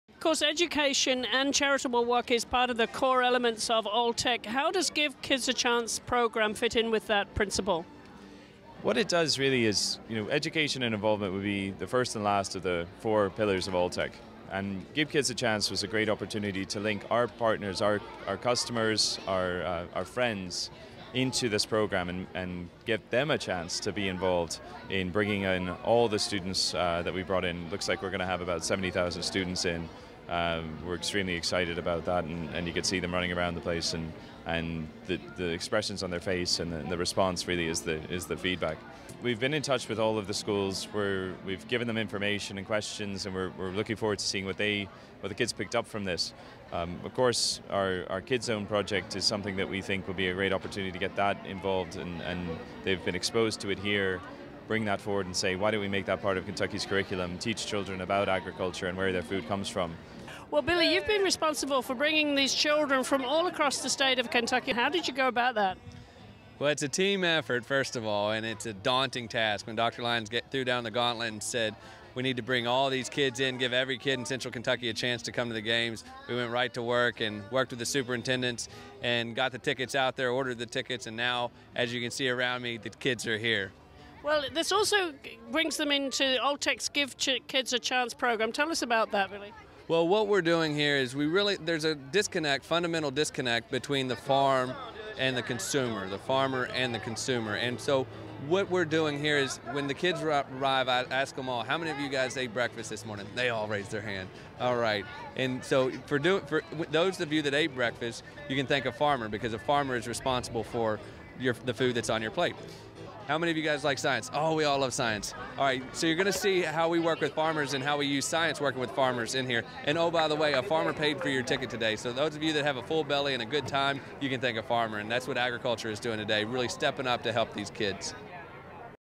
Giving Kids a Chance
Alltech and its feed industry partners launches a program enabling schoolchildren from Kentucky to visit the Alltech FEI World Equestrian Games to enjoy the event but also learn about agriculture.